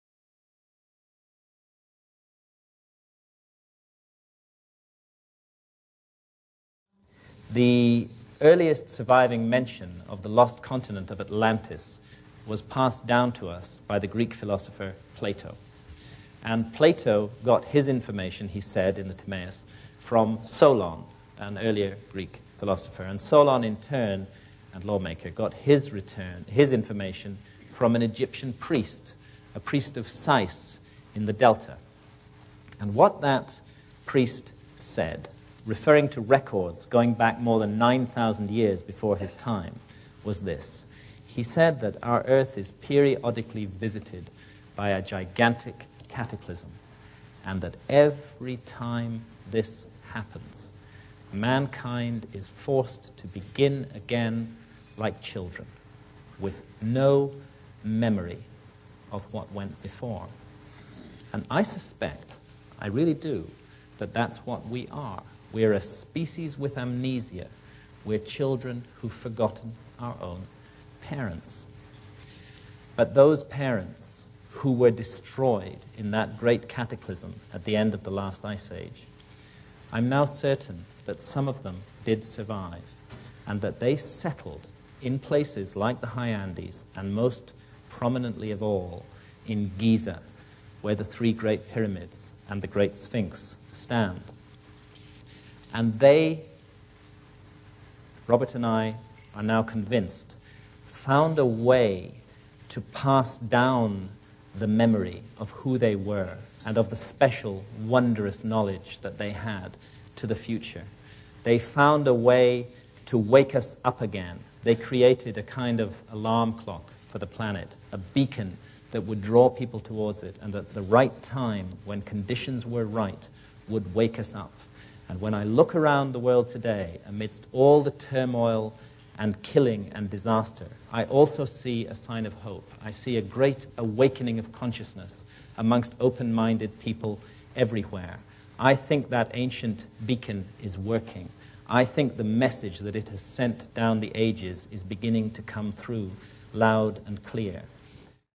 Graham Hancock ~ A Species With Amnesia ~ Atlantis
Author Graham Hancock traces what we know about Atlantis, and how we have lost touch with our ancient past.